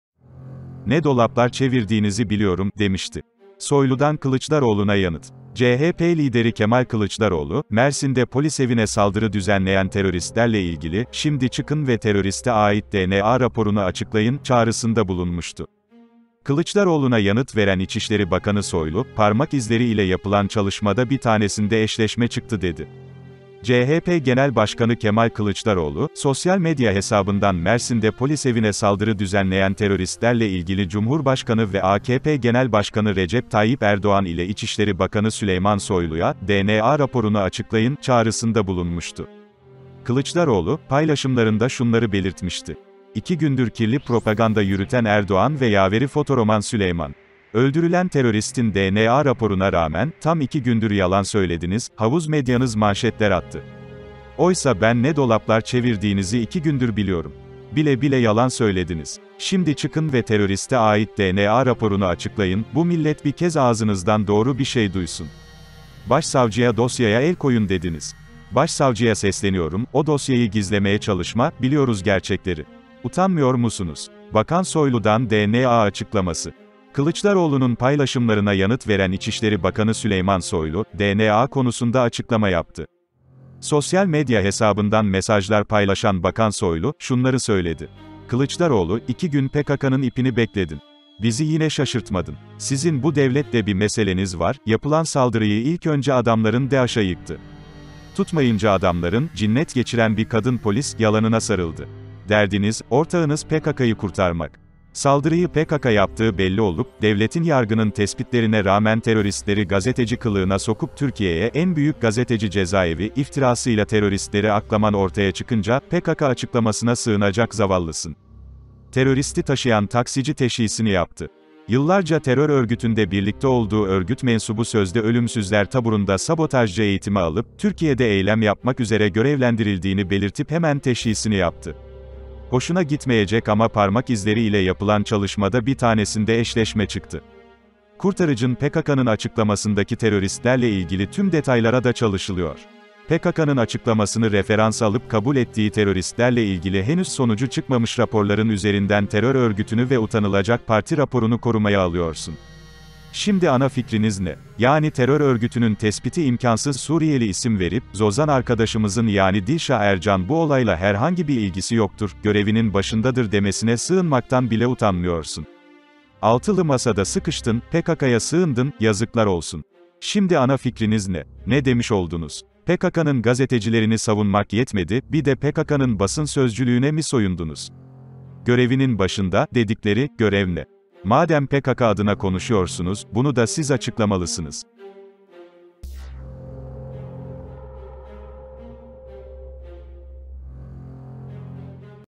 Soylu'dan Kılıçdaroğlu'na Zehir Zemberek Sözler: Derdin Ortağunuz PKK'yı Kurtarmak
Ne dolaplar çevirdiğinizi biliyorum’ demişti... Soylu'dan Kılıçdaroğlu'na yanıt
CHP lideri Kemal Kılıçdaroğlu, Mersin'de polisevine saldırı düzenleyen teröristlerle ilgili, "Şimdi çıkın ve teröriste ait DNA raporunu açıklayın" çağrısında bulunmuştu. Kılıçdaroğlu'na yanıt veren İçişleri Bakanı Soylu, "Parmak izleri ile yapılan çalışmada bir tanesinde eşleşme çıktı" dedi.